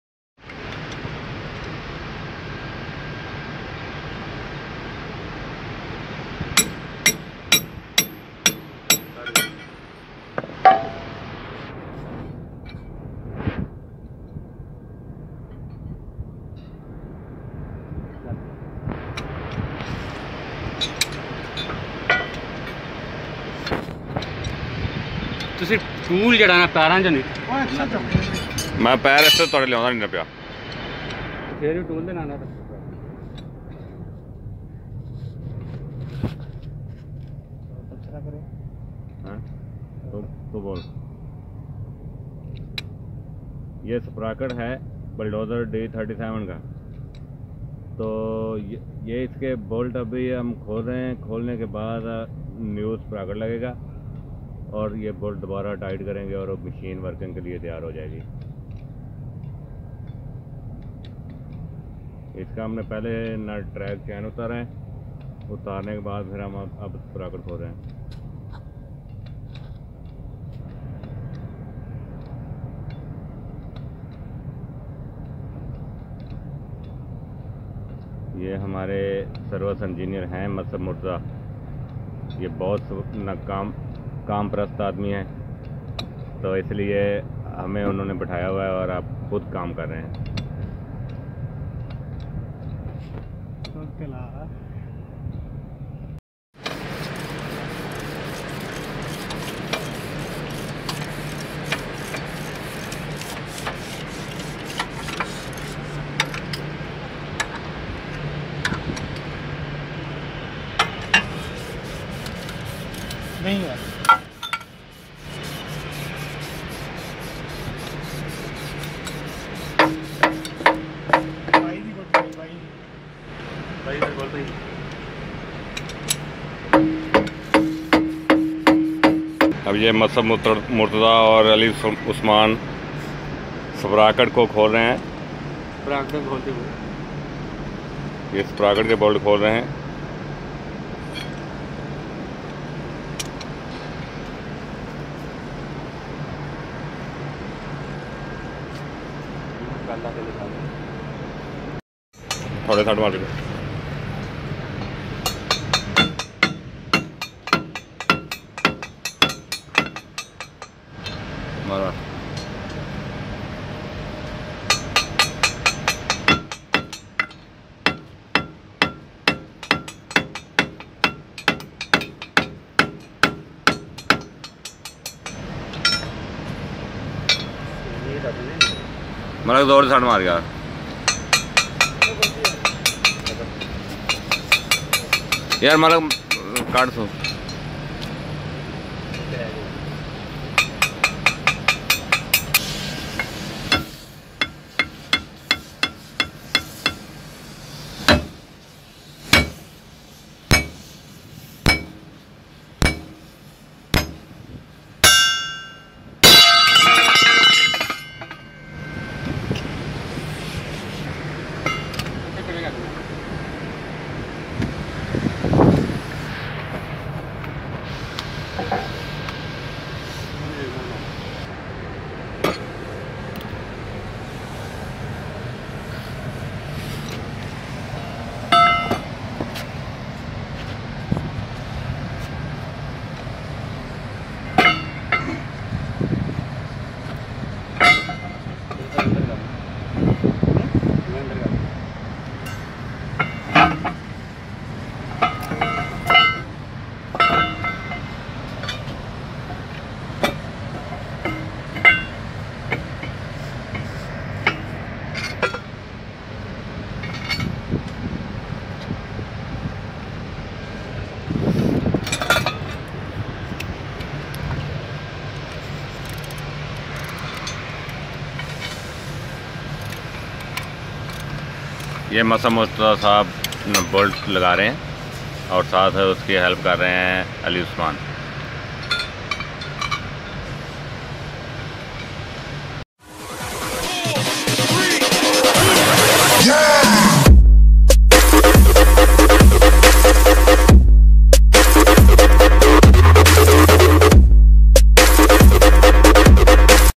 How to replace sprocket wheel of Komatsu dozer
Sprocket wheel installation
KOMATSU DOZER D37EX-22
Technical Video